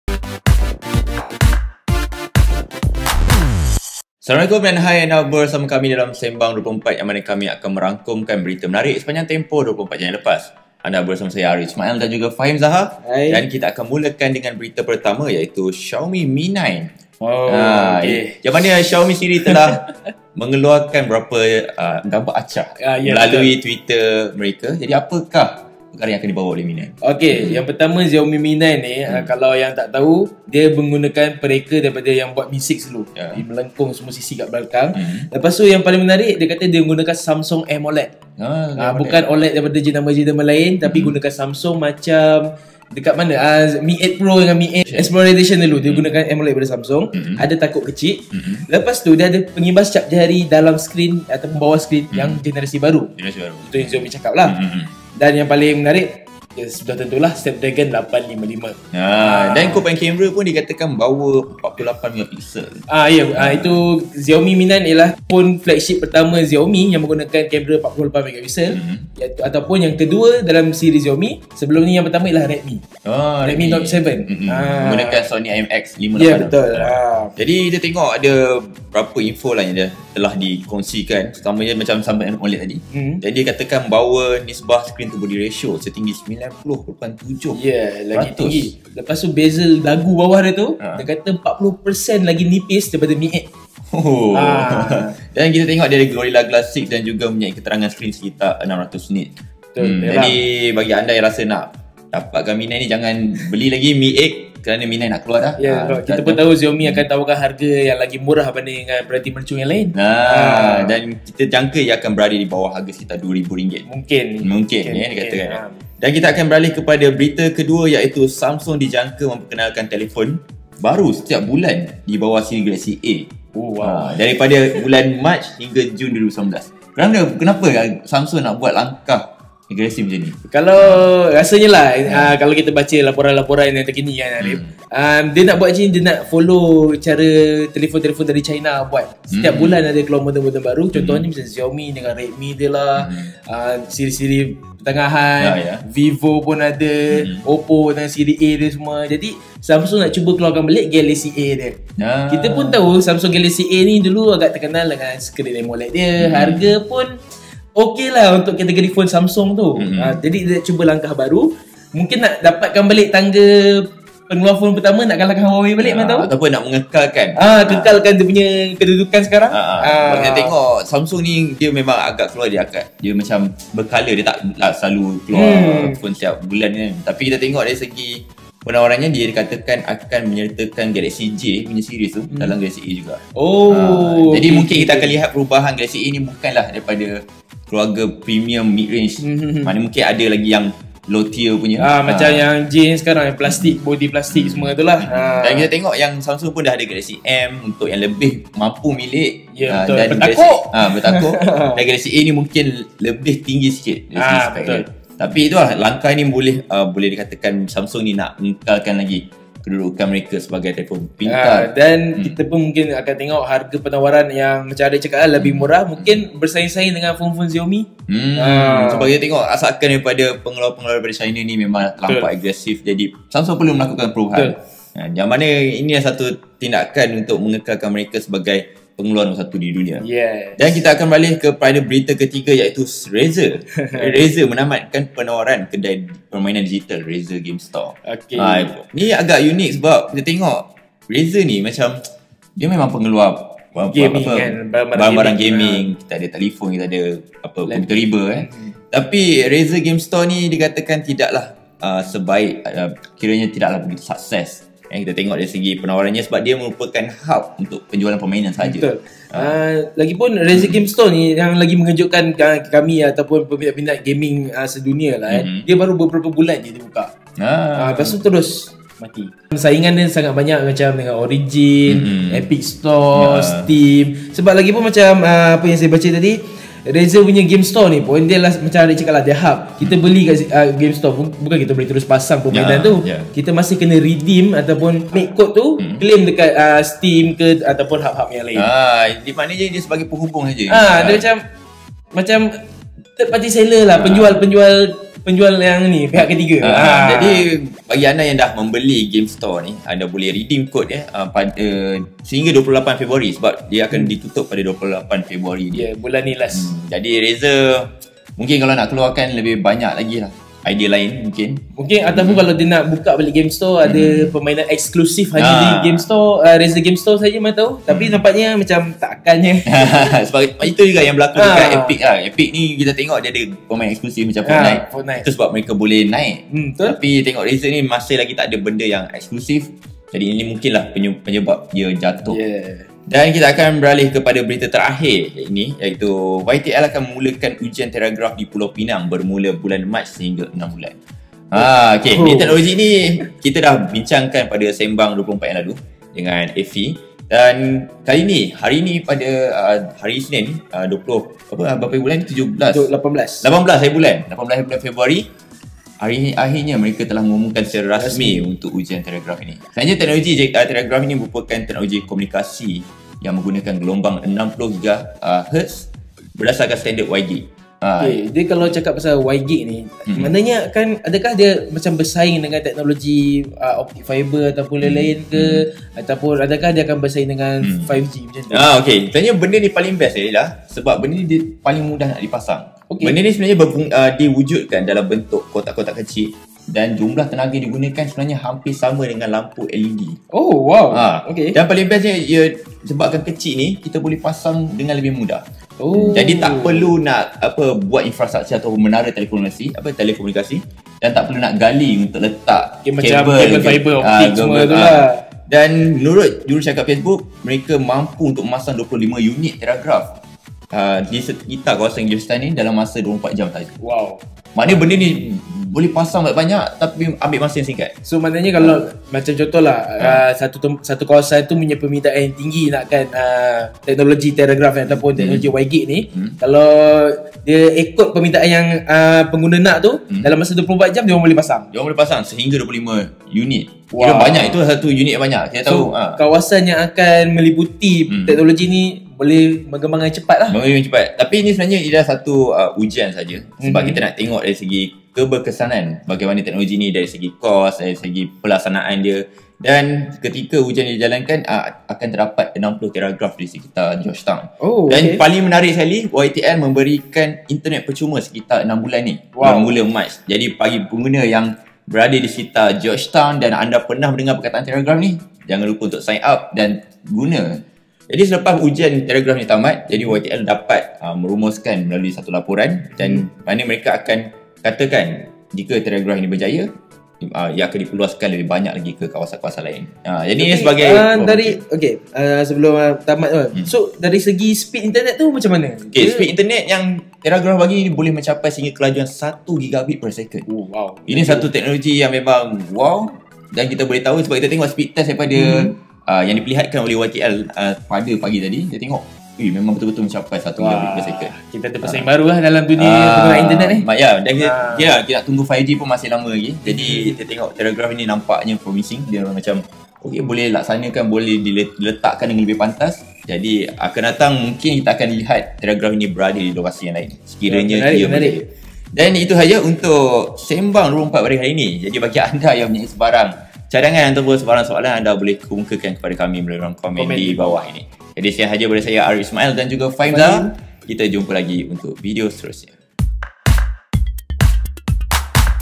Xiaomi Mi 9 - Spesifikasi Dan Informasi Awal
Xiaomi kini telah mengeluarkan beberapa informasi dan gambar awal untuk telefon pintar akan datang mereka, Xiaomi Mi 9. Mereka telah sedia memperlihatkan rekaan bahagian hadapan dan belakang, selain berkongsi sokongan kamera padanya. Selain itu, di Pulau Pinang, YES dan Facebook telah memulakan ujian Terragraph, menawarkan kelajuan internet sehingga 1Gbps melaluinya.